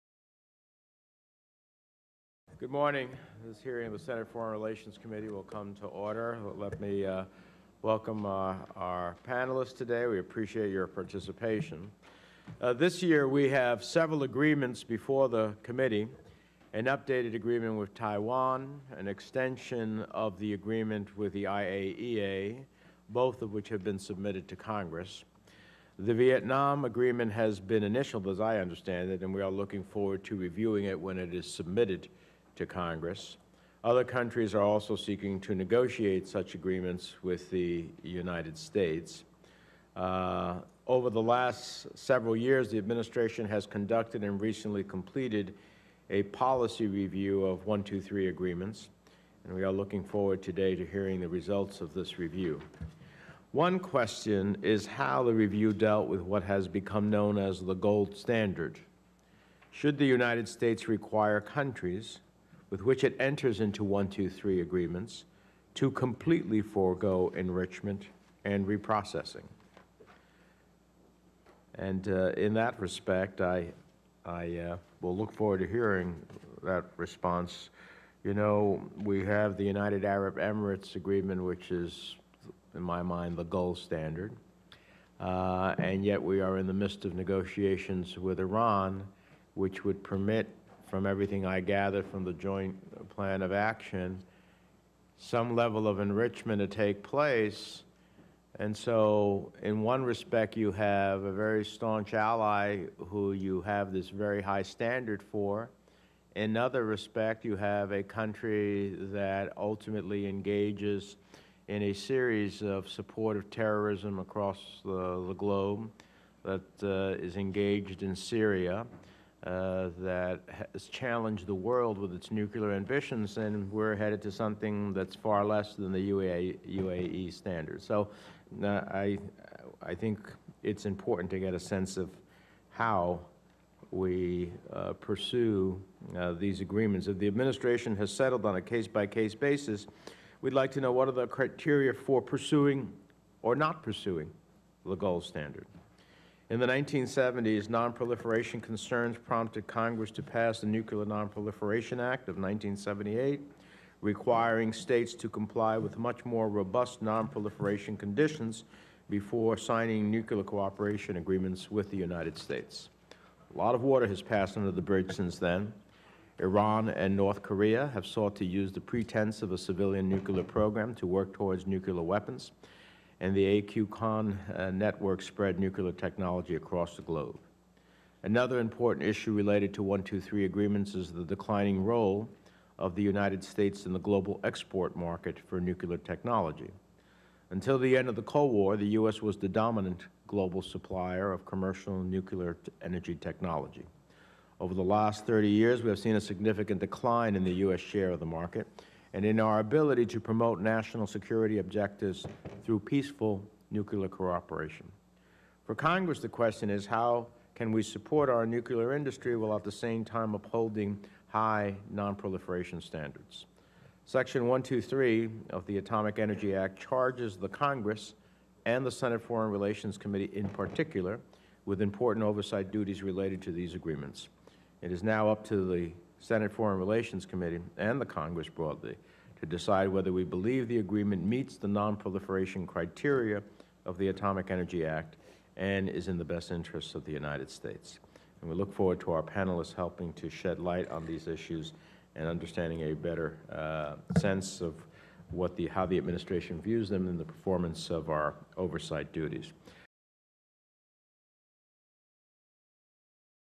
Chairman Menendez Opening Remarks at Hearing on Civilian Nuclear Cooperation Agreements
Senator Bob Menendez, Chairman of the Senate Foreign Relations Committee, provides opening remarks at a hearing on civilian nuclear cooperation agreements.